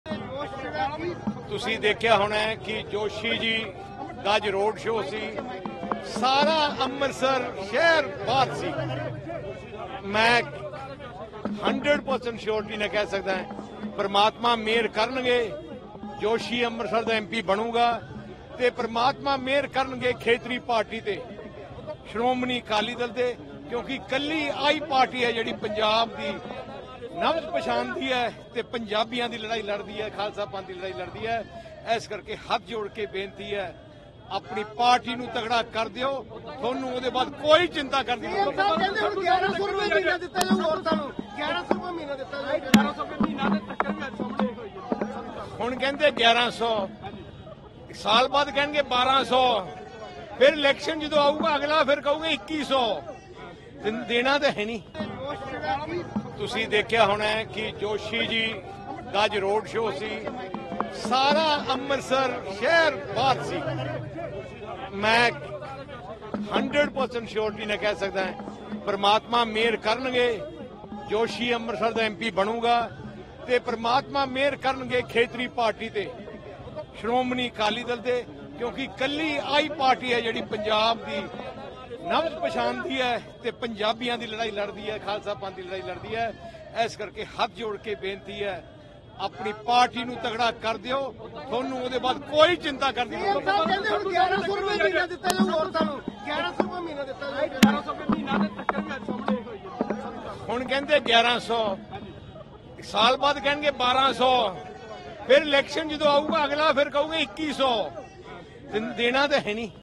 “Anil Joshi will become the MP..." SAD chief Sukhbir Badal exudes confidence over Amritsar seat
“Anil Joshi will become the MP..." SAD chief Sukhbir Badal exudes confidence over Amritsar seat in LS Polls